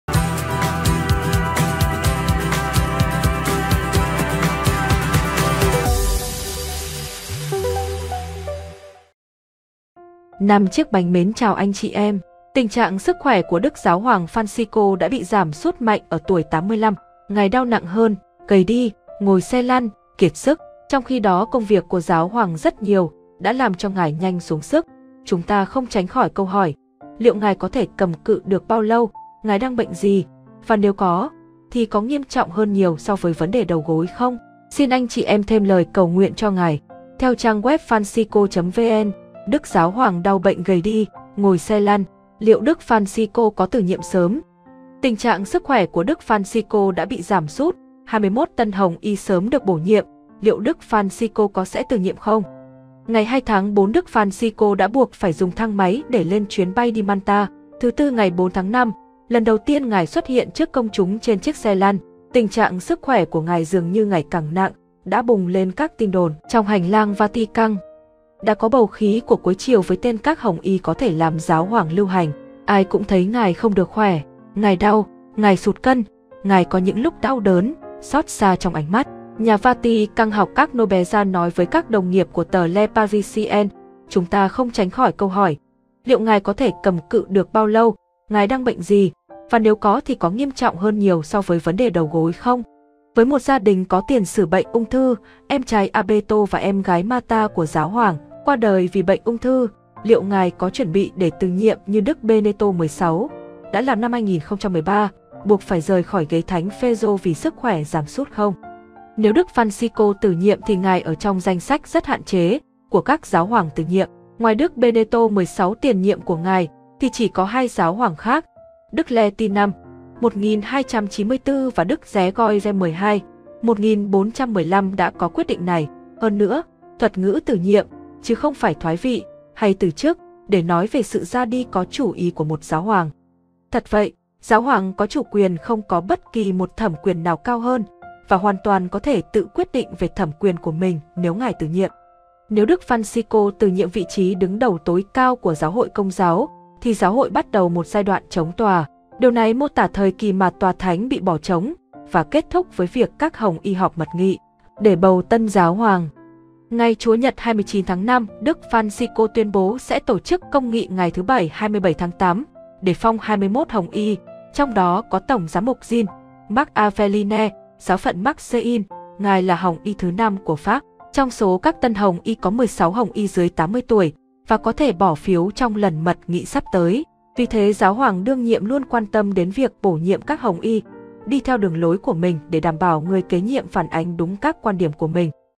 🔴Tin Mới: Xót Xa Về Đức Giáo Hoàng Ngay Lúc Này - Xin Cầu Nguyện Đức Thánh Cha Phanxicô
Tình trạng của Đức Giáo Hoàng Phanxicô đã bị giảm sút mạnh ở tuổi 85. Xin anh chị em thêm lời cầu nguyện cho Ngài.
Xin Cầu Nguyện Đức Thánh Cha Phanxicô